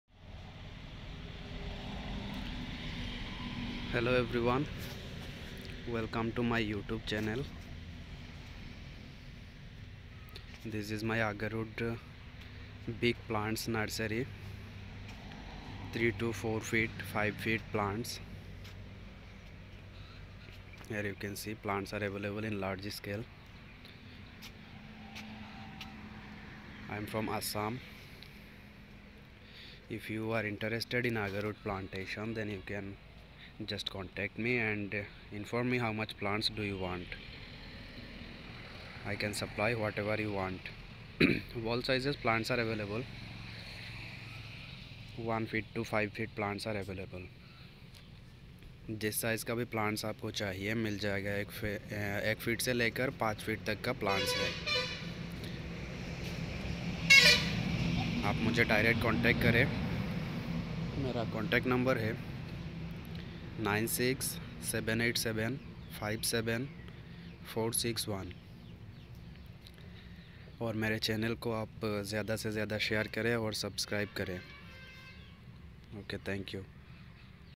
Agarwood plants in Assam | Agarwood tree plantation | Call-06000371241..
Agarwood plants Nursery in my Nursery in Assam.. Here in this video you can see small size and big size Agarwood plants 1 feet to 5 feet Agarwood plants available.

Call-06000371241

Nursery: Assam
Scientific name: Aquilaria
Variety: Khasiana and Malaccencis
Delivery possible: All over India
Size of plants: 1 feet to 5 feet